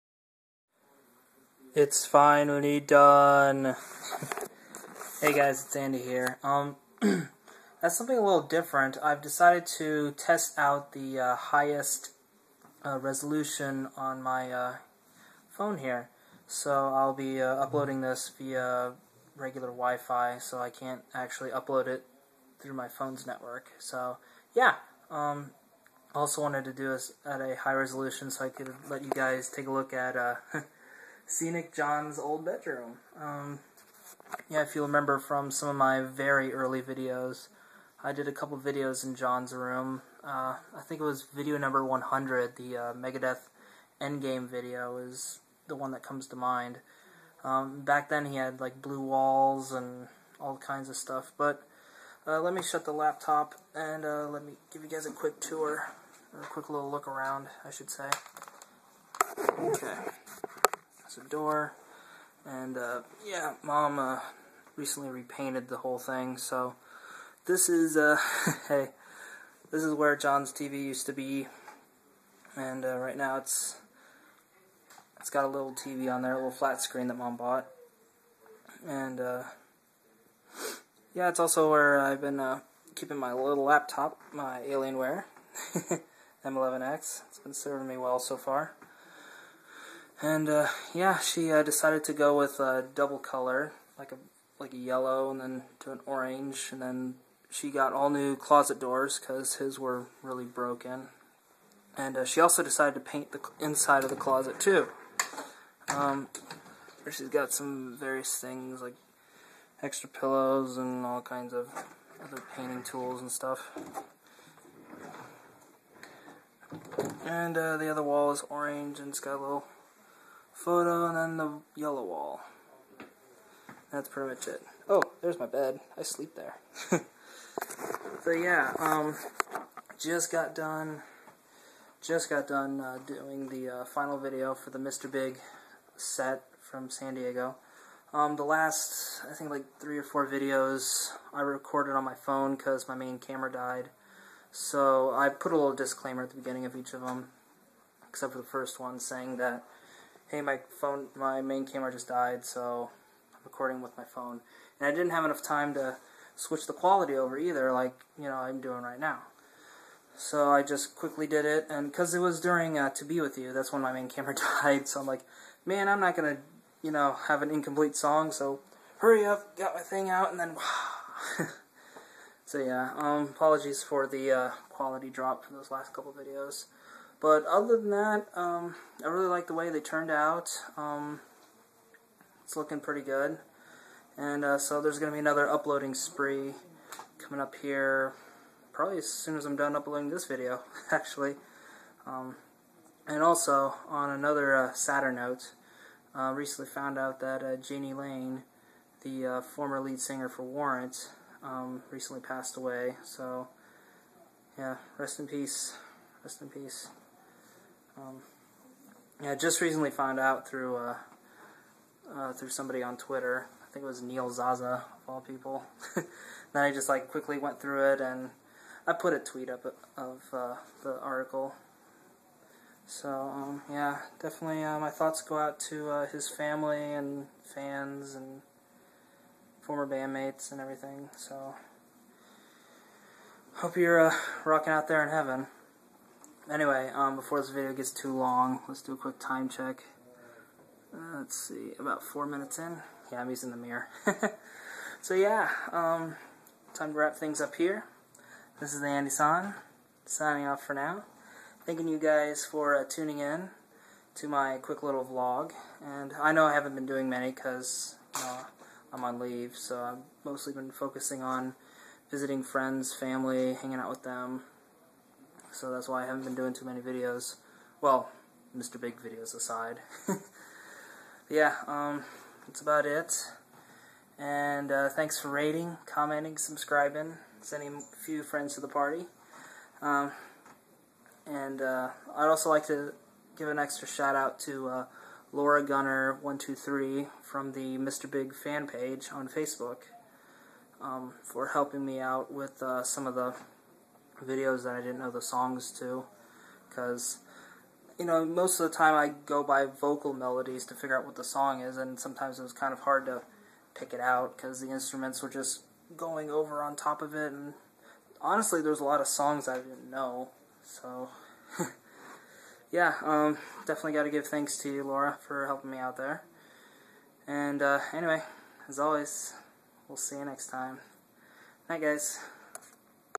RIP Jani Lane & August 2011 On-Leave Update - Vlog 086 - TheAndySan
In this high-quality mobile vlog, I give you guys a quick tour of Jon's old room and talk about finishing the Mr. Big videos, the recent passing of former Warrant singer Jani Lane, and giving a special shout-out to laragunner123 of the Mr. Big fanpage on Facebook for helping me name some of the songs in the set list. Enjoy!!

TheAndySan